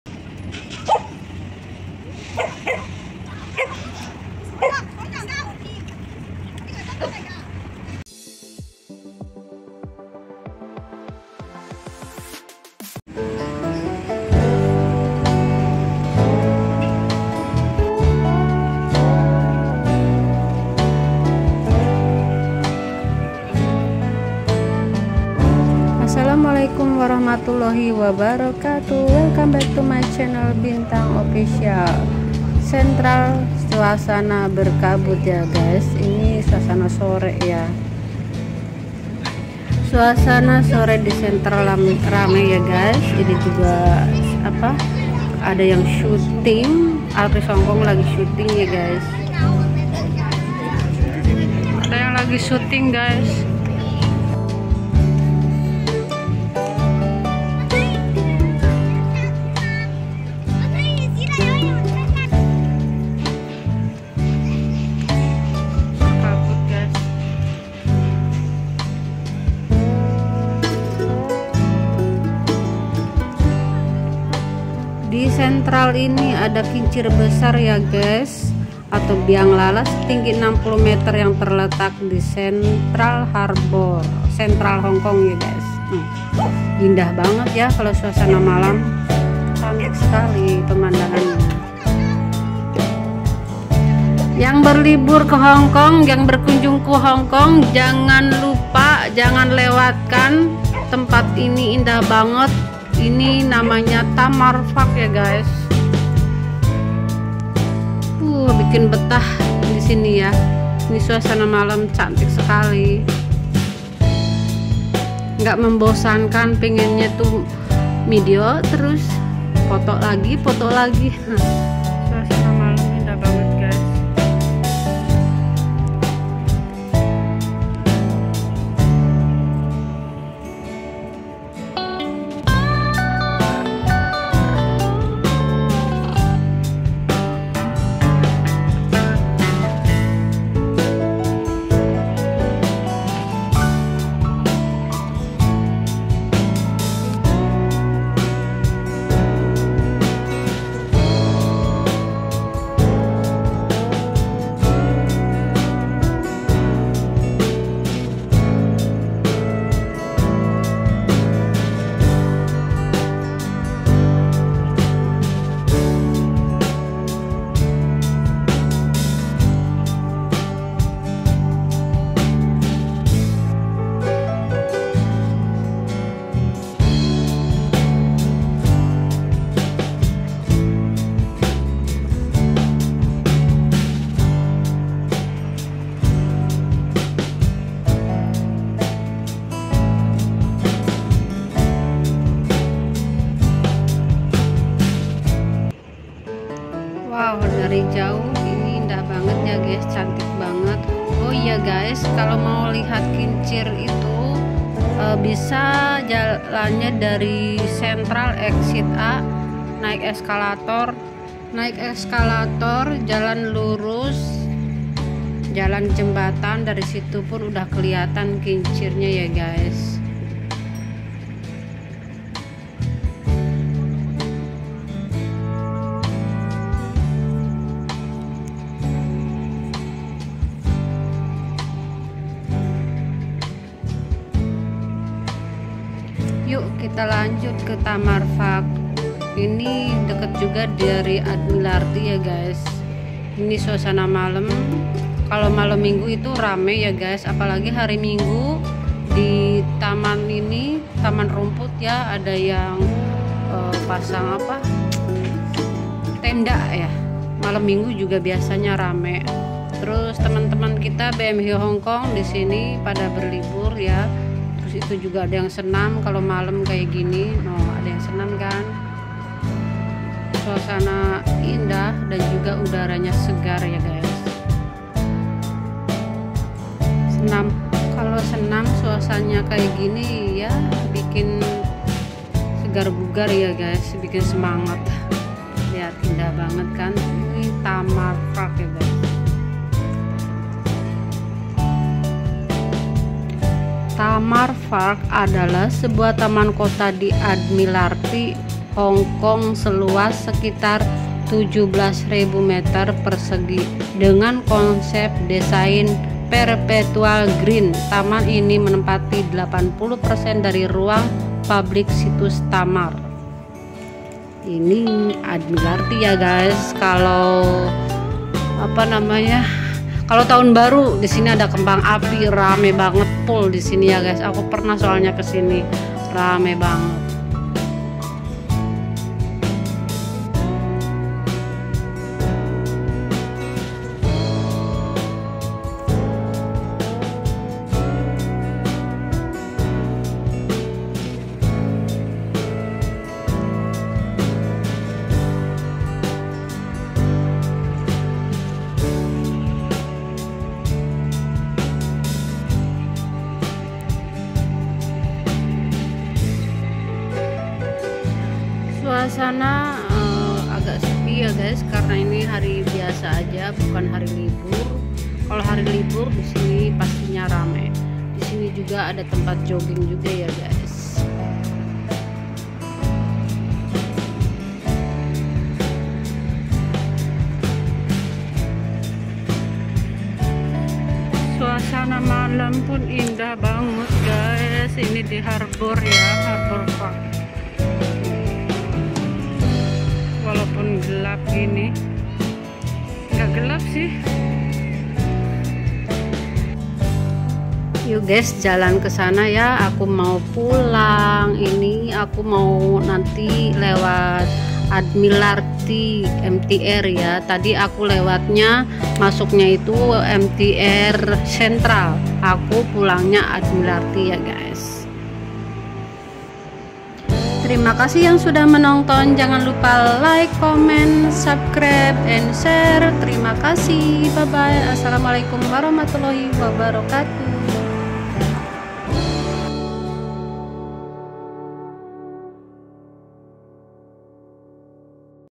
Liburan Ke Hong Kong Jangan Lewatkan Tempat ini || Tamar Park Admilarty
Liburan ke Hong Kong jangan lewat kan tempat ini,Tamar Park Admilarty.
Rute,central Exsit A naik eskalator jalan lurus sebelah kanan sudah kelihatan tempatnya atau Admilarty Exsit A
HONG KONG observation wheel setinggi 60 meter yang terletak di central harbourfront,central, Hong Kong memiliki 42 gondala.dan di sebelah nya ada Tamar Park taman kota di Admilarty, Hong Kong seluas sekitar 17.000 meter persegi ya guys.